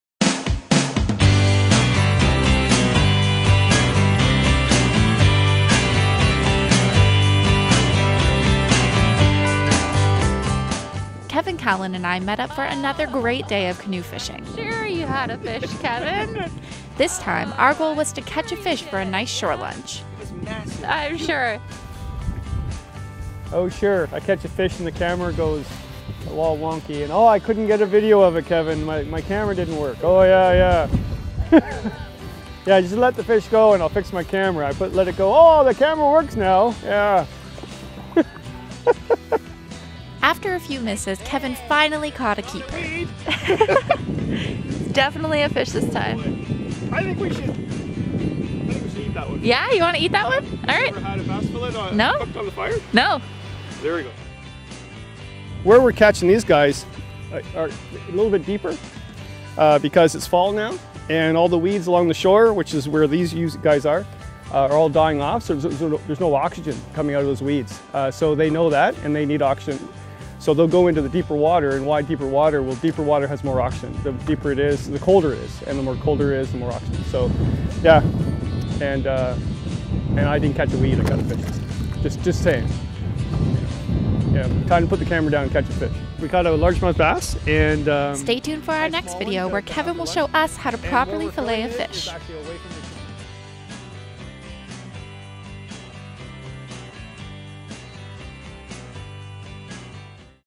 Fall Fishing with Kevin Callan
Sarah and Kevin Callan, otherwise known as the Happy Camper, went fishing again on Kevin's favourite bass lake. This time, they tried to catch a fish for their shore lunch.